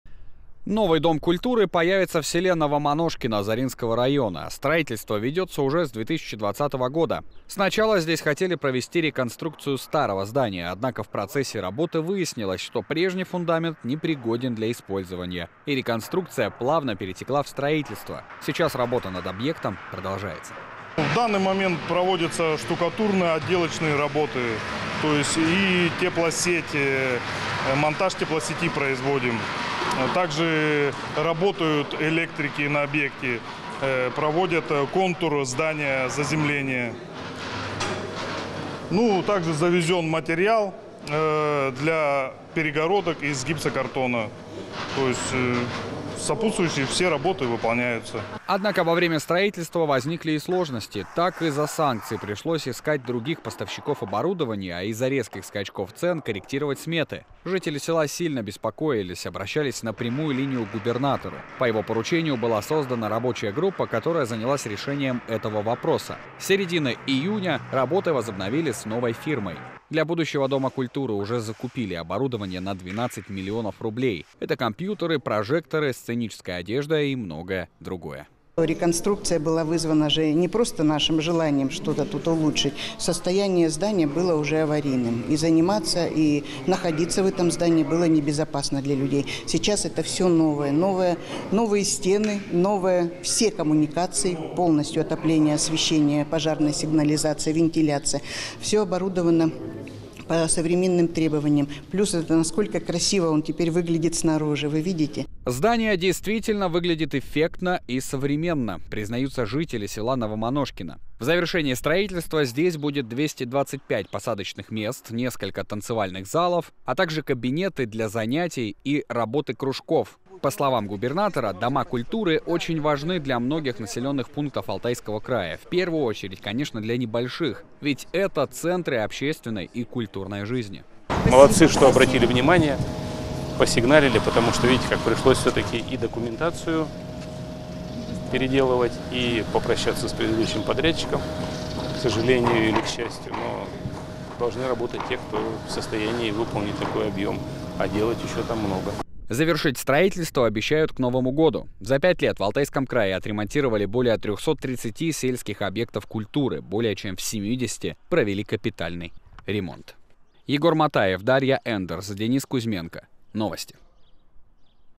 Новый дом культуры строят в селе Новомоношкино Заринского района
Губернатор Алтайского края Виктор Томенко лично проверил, как протекает ход работ.